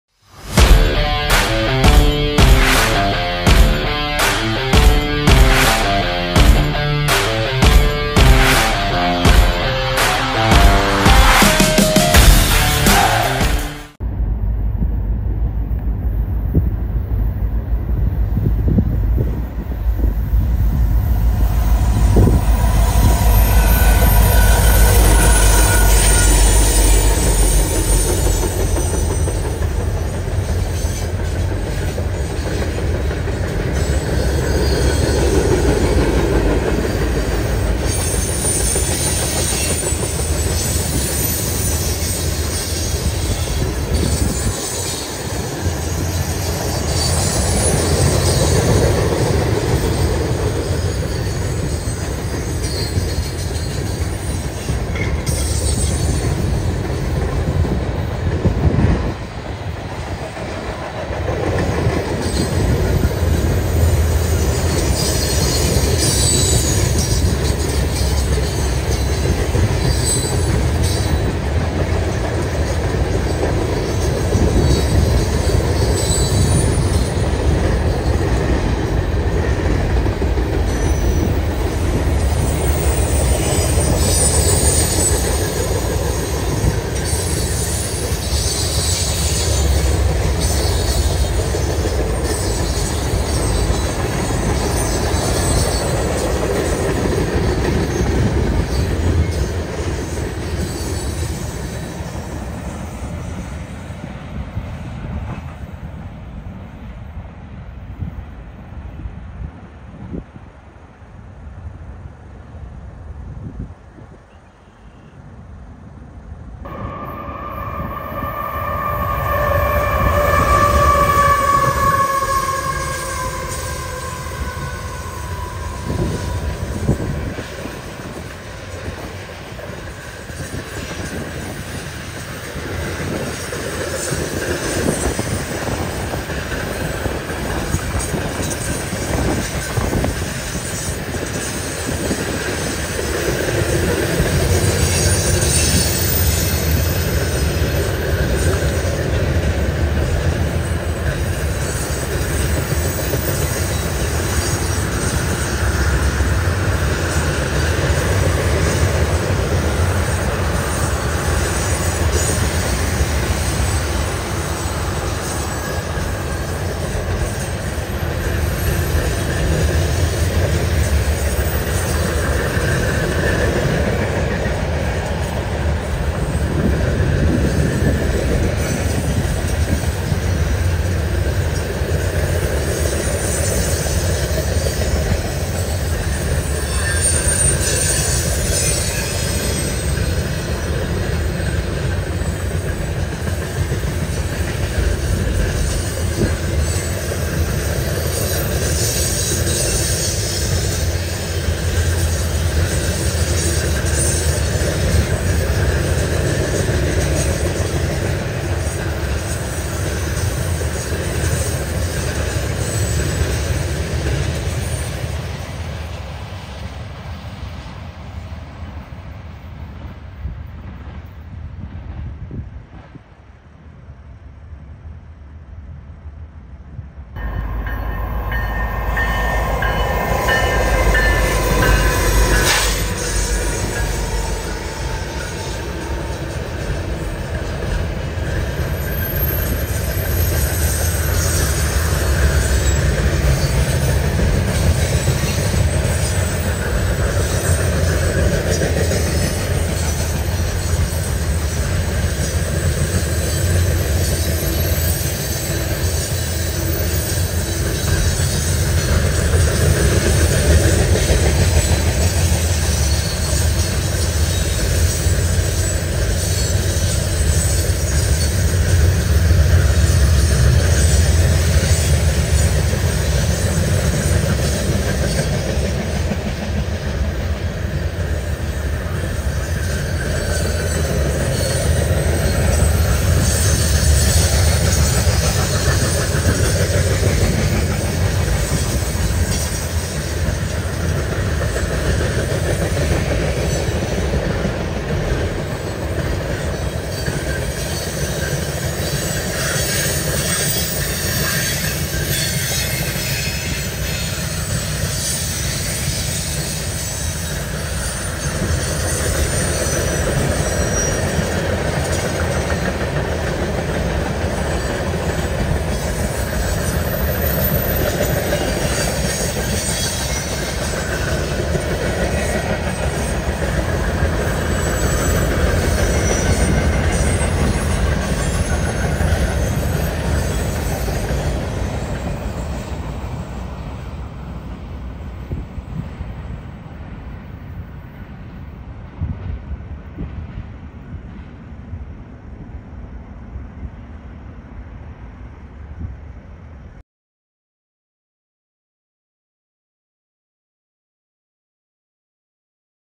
Canadian National Freight Trains in Plainfield
While railfanning in Plainfield, I caught 3 Canadian National trains in Plainfield.  All 3 Canadian National  freight trains were rumbling south through Plainfield.